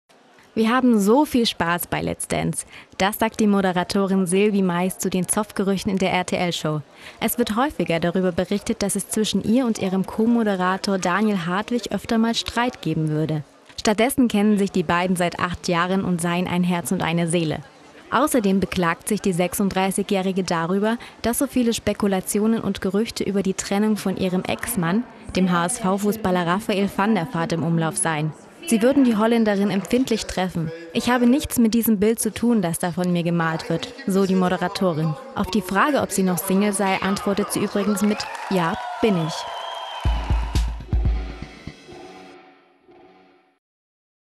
Sylvie Meis: Kein Streit bei «Let's Dance»
Zwietracht bei der RTL-Show «Let's Dance»? Nein, wehrt Moderatorin Silvie Meis (36) ab. «Wir haben so viel Spaß bei "Let's Dance"», sagte die Niederländerin in einem Gespräch mit der Zeitschrift «Grazia». - Entertainment pur! Her findest Du die neuesten Infos zu Deinen Stars!